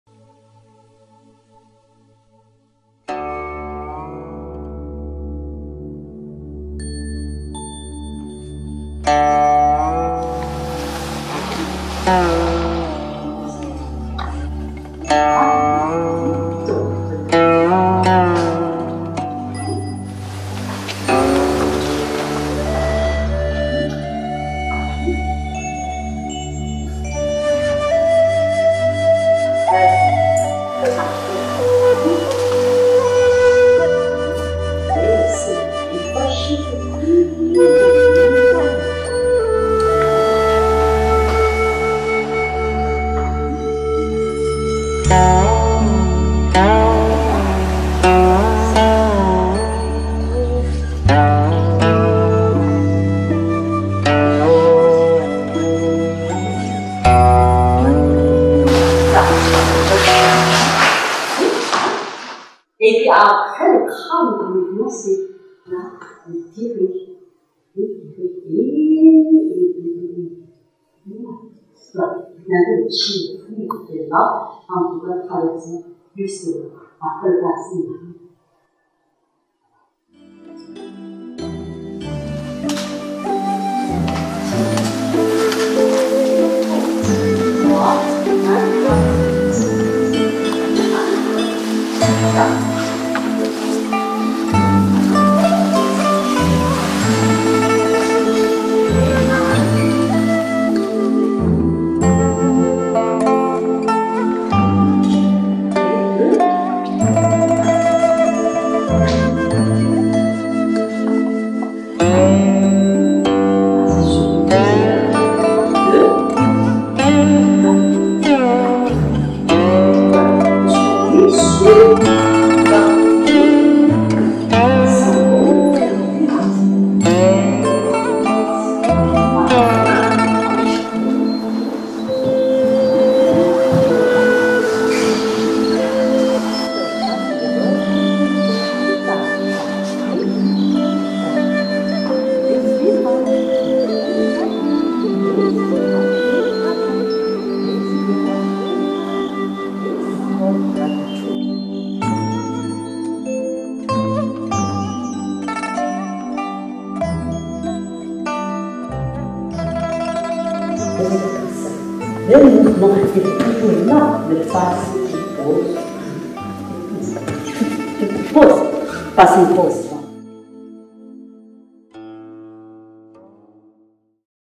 "Le travail du Dragon" avec maître Ke Wen et les élèves des Temps du Corps
Extraits de la formation à la culture taoïste et aux arts énergétiques de Wudang.  Les cours sont animés par les  maîtres  Yuan Li Min, Qing Feng Zi, Ke Wen et par Sophie Faure et Liu Cui Fang.